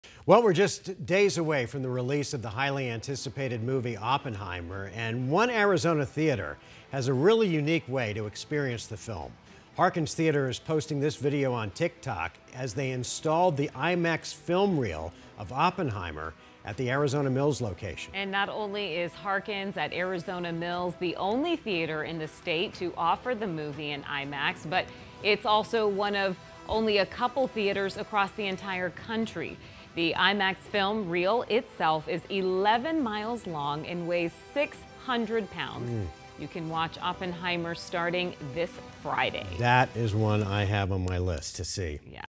Watch Harkins Theatres install IMAX film reel ahead of Oppenheimer debut
The Arizona Mills Harkins is the only theatre in the state to offer Oppenheimer in IMAX, and one of the only theatres in the country.